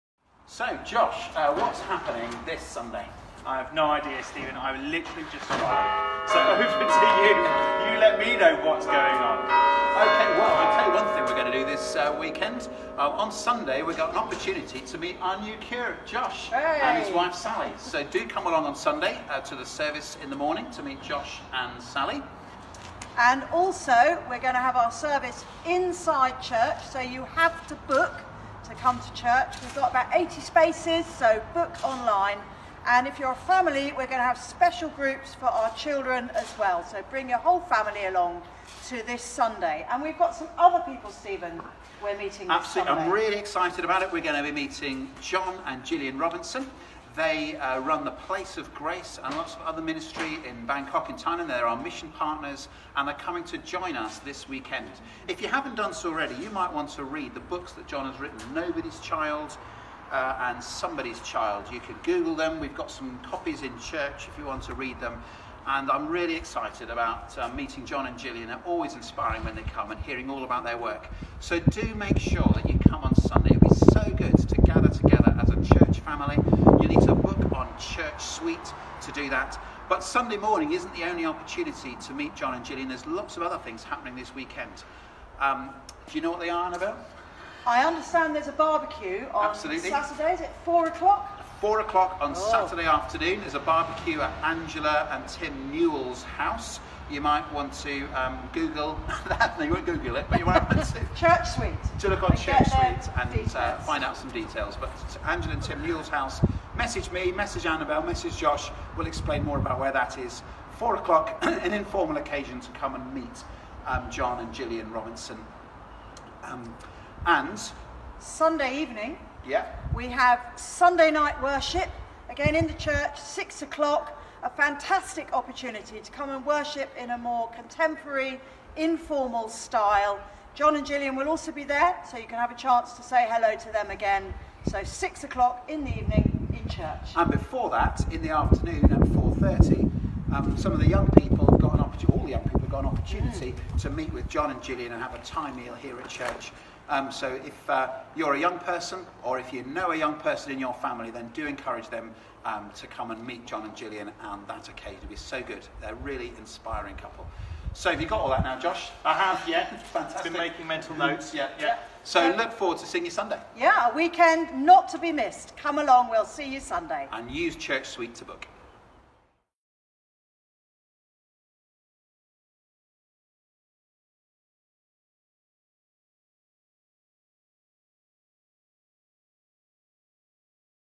Trailer
We have lots going on this weekend! Here is a short video telling you what's happening from Stephen Annabel and our new Curate Josh!
Join together live at 10:30 to worship in the church building!
To make sure you get a place book on church suite.